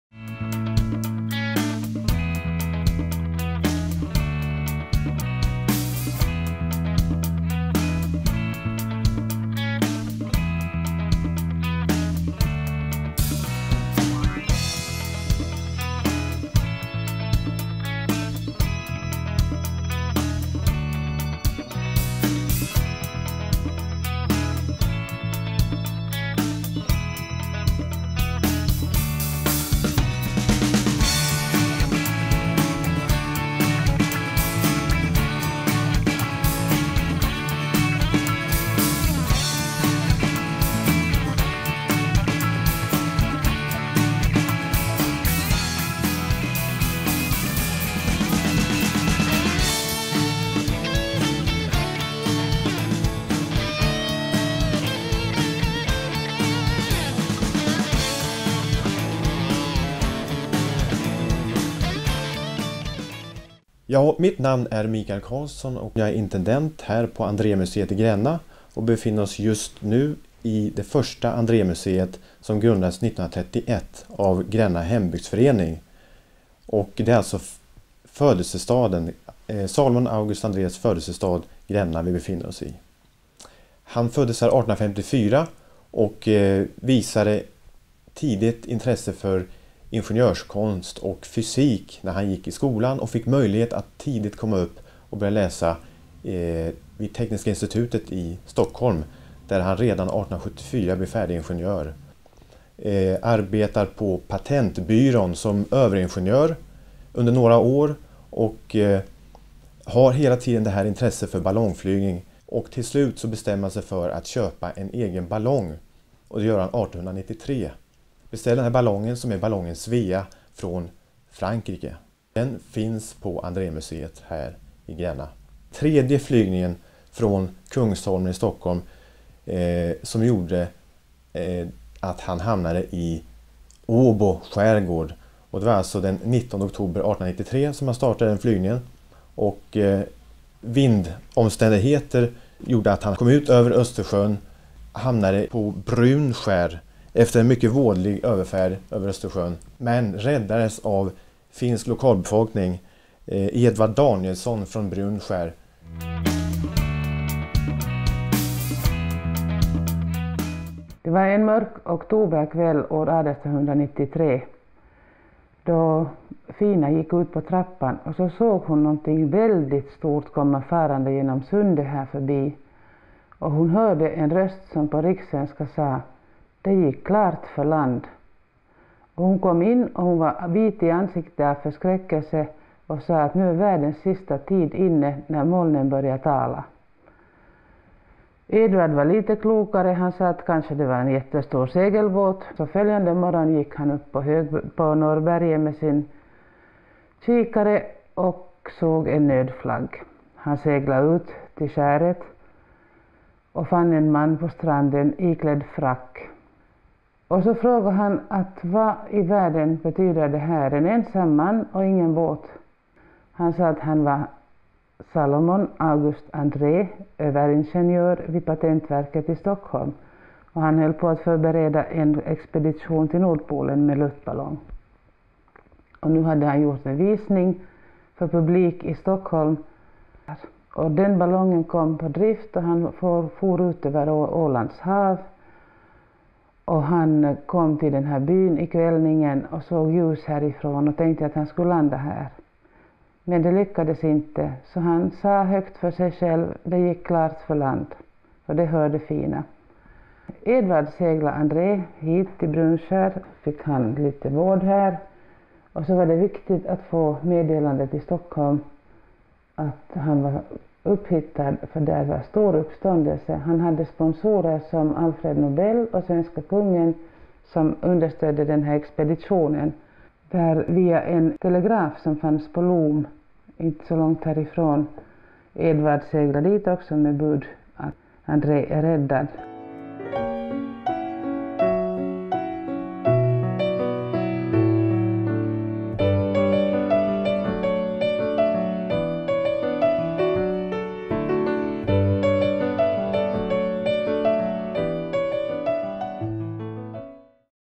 Grenna, Sweden
Grenna Sweden The town is situated at the foot of Gränna mountain and is characterized by its steep streets and old wooden houses. Gränna is known for its red and white polkagris (literally "polka pig" in Swedish) stick candy, also known as peppermint rock.
The balloonist Salomon August Andrée, who died in an attempt to reach the North Pole by balloon, was born in Gränna.
The Grenna Museum hosts an exhibition of the expedition together with a comprehensive collection of related objects and photos.